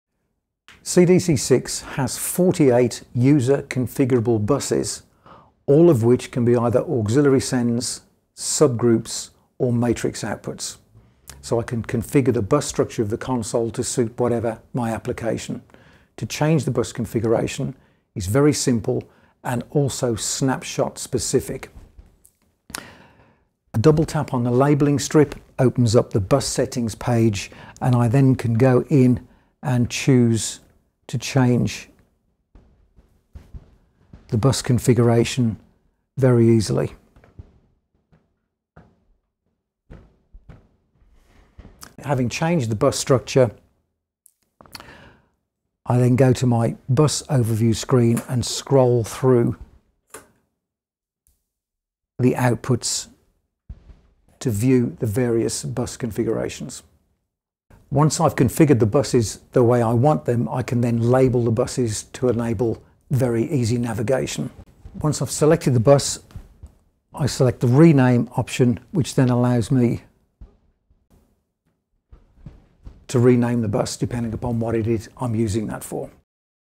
Cadac CDC six: Configurable Busses
This video provides an overview of the configurable busses on the Cadac CDC six.

Overview:
• 48 configurable output busses
• Assignable as Groups, Stereo Groups, Auxes, Stereo Auxes or Matrixes
• User definable naming

Cadac, based in the UK, design and engineer digital and analogue audio mixing consoles for concert touring, theatres, worship facilities and live venues.